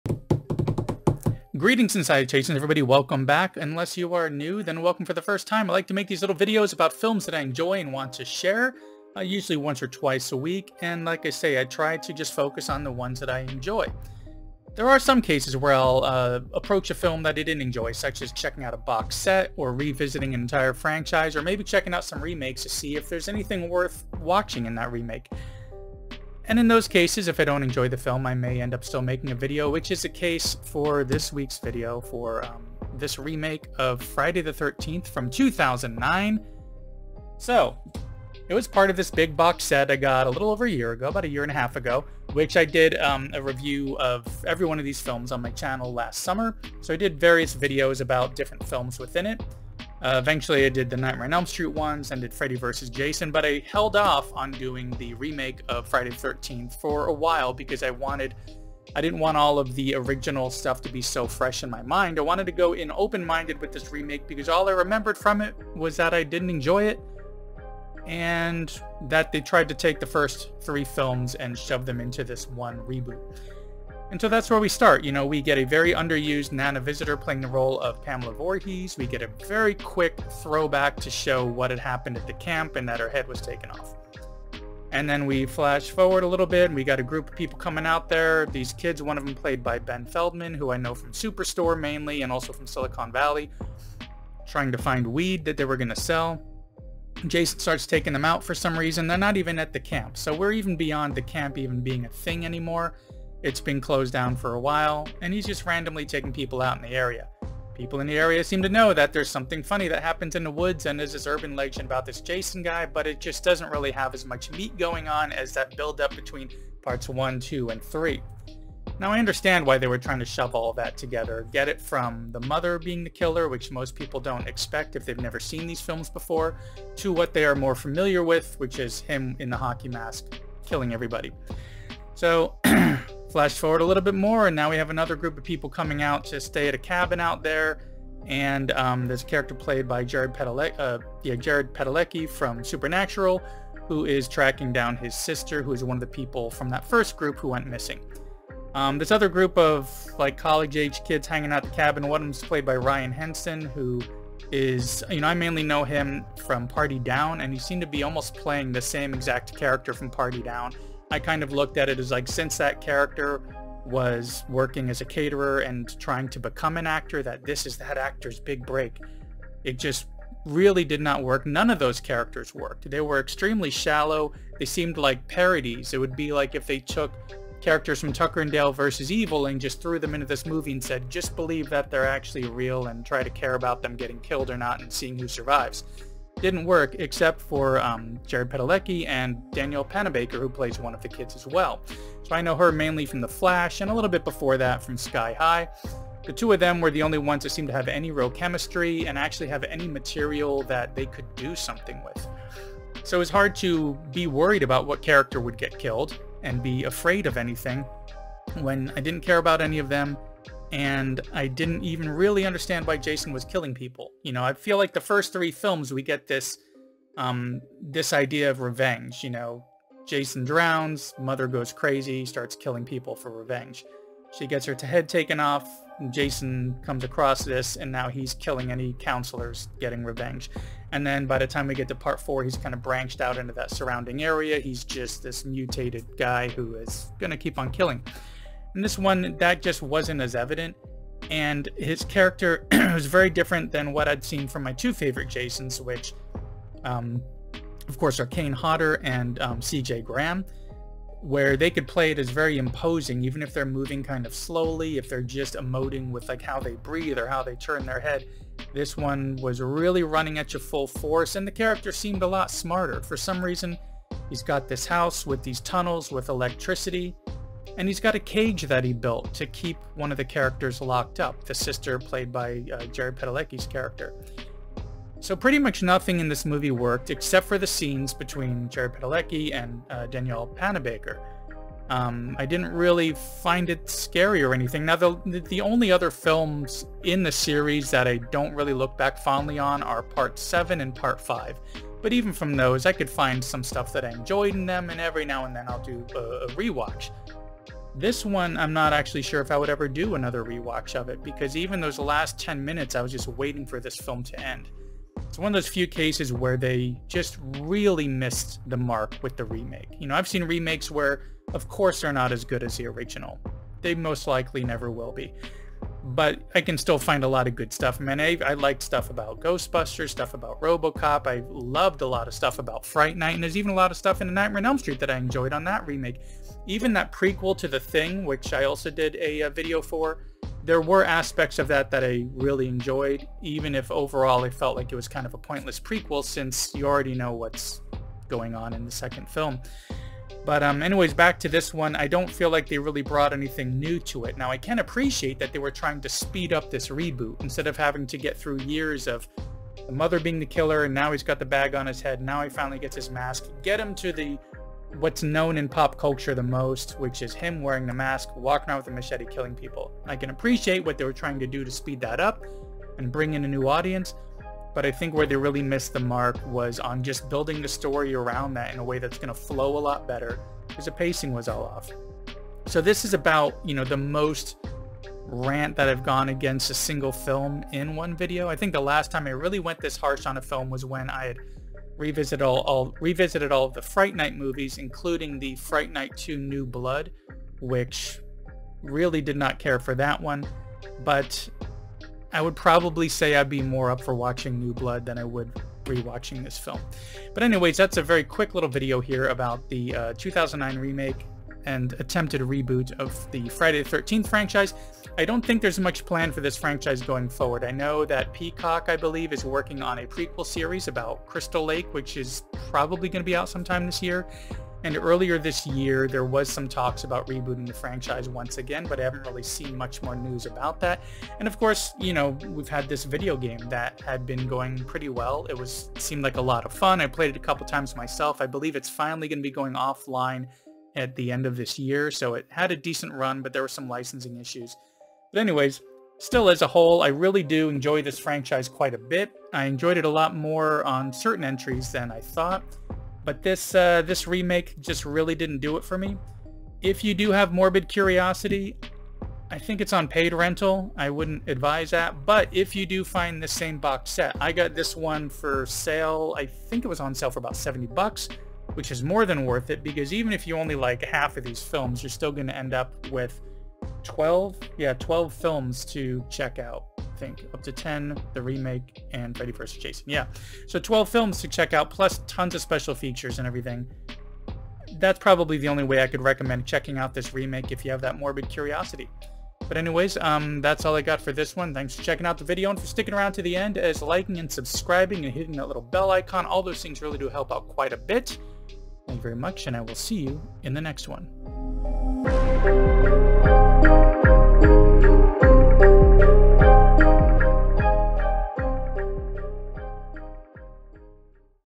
Friday the 13th (2009) - Remake Review / Franchise Revisit [Missed the Mark]
Unlike one of my previous Remake Reviews, Evil Dead (2013), this one really missed the mark for me.

Friday the 13th Deluxe Edition: